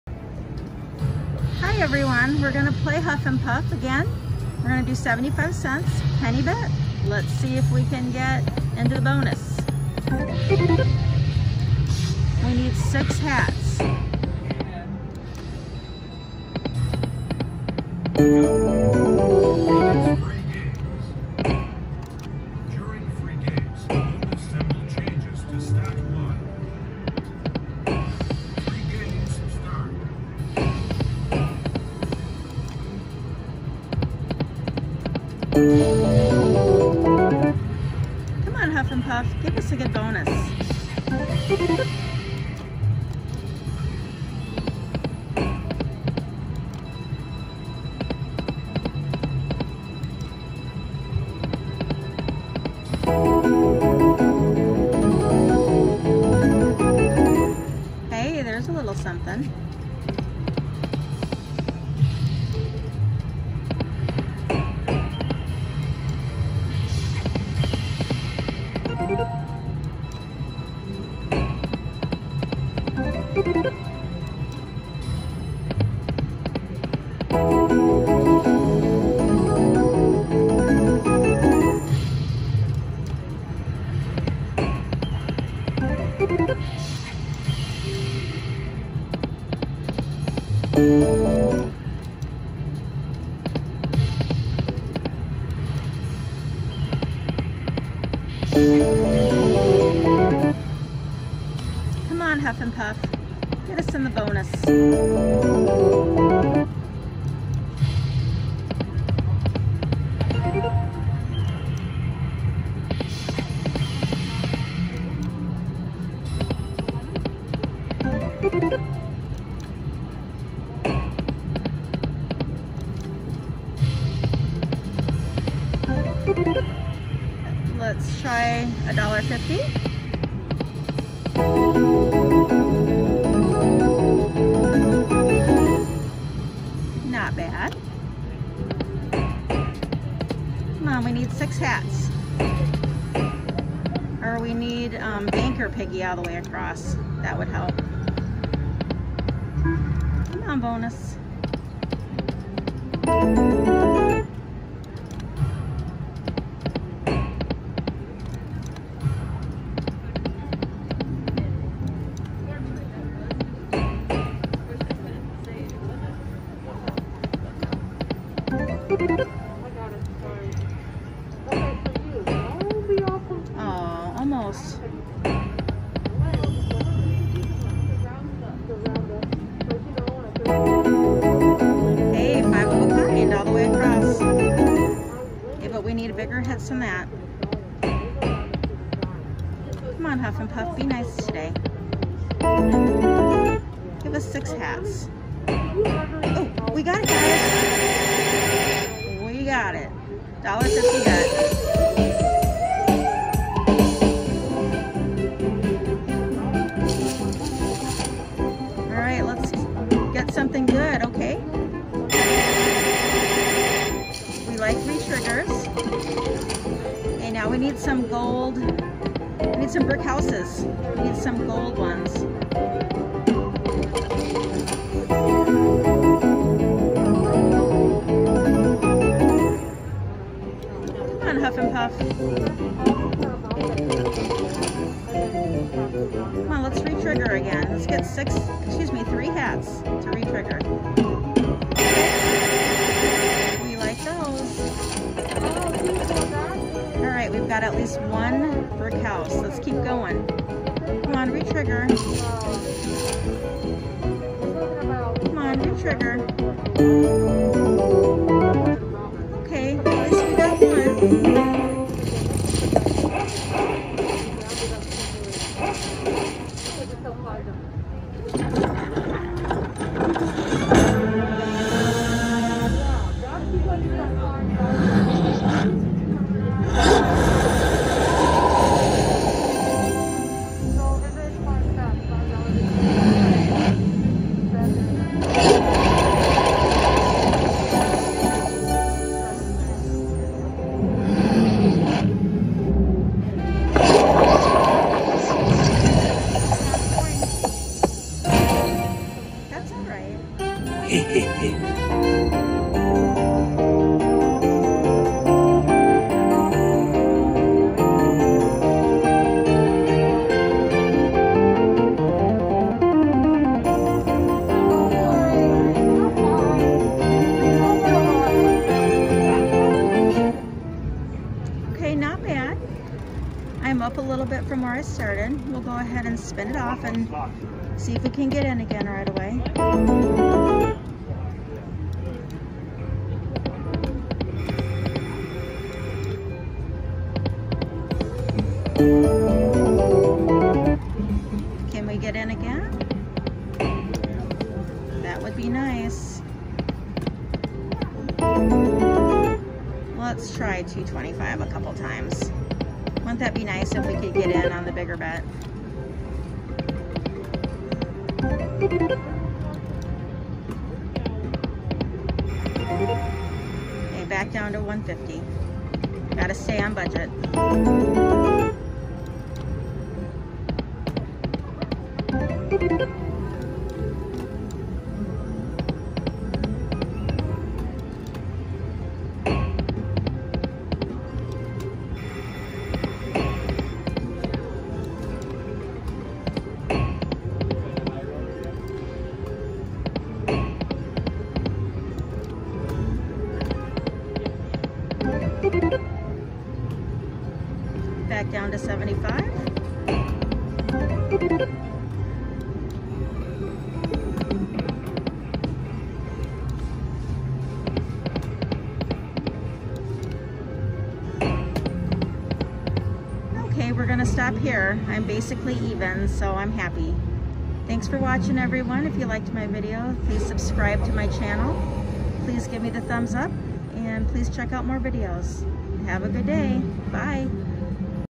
I Asked for a Bonus and I Got a Bonus! Huff N Puff
Lock It Link Huff N Puff
Playing pennies.  .75, $1.50 & $2.25 bets
I get the bonus and it gives me a coin!
If you enjoyed one of my videos, please like it by giving me a thumbs up 👍 and commenting.